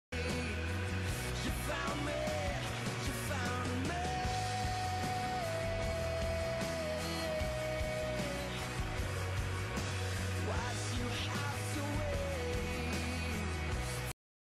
Found me VENT and MEP part 15.wmv
PLEASE DON"T ASK THOUGH COMMENT WHATS WRONG! FOR THE FEW THAT NOW CARE ABOUT MY LIFE SEND ME A PM

Now thats out of the way.

I feel like the biggest failure in the planet.  Why do i deserve anything? Anything at all? Now i know someone is going to cheer me up. I don't deserve it. But who ever does. thank you.

This is also a MEP part. I really wanted this to be awesome. thats why there is so much masking.

Story-Xion is slowly suffring but Zidane her best friend is too busy on his love life to notice her. When ever they was around all Zidane could talk about was how in love he was with Dagger. Not realized that something was bothering Xion. It was not until he found her in misery and telling him to hate her that he realized some thing was wrong. But by then it was too late. she died.